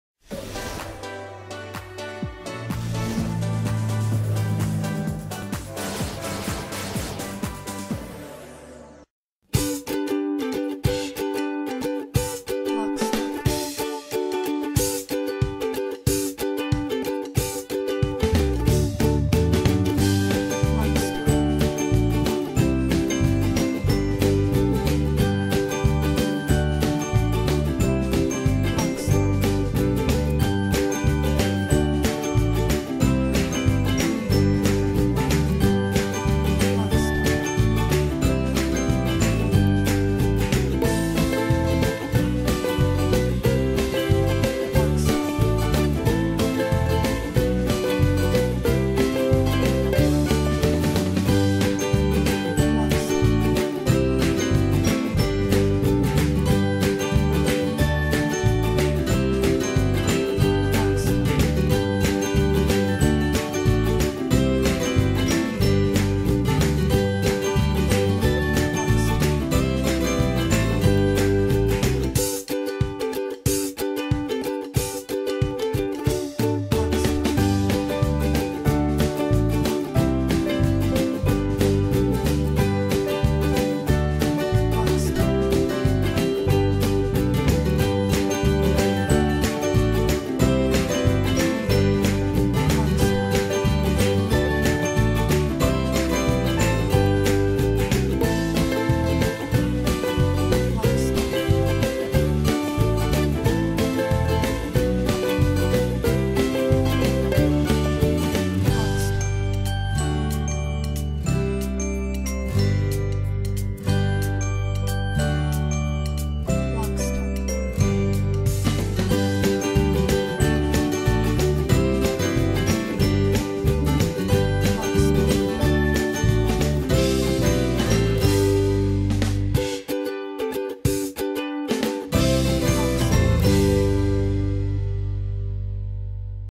Kids Wonderland Science Camp Trailer
From thrilling adventures to whimsical world, Kids Wonderland is packed full of fun science adventures for your child to explore!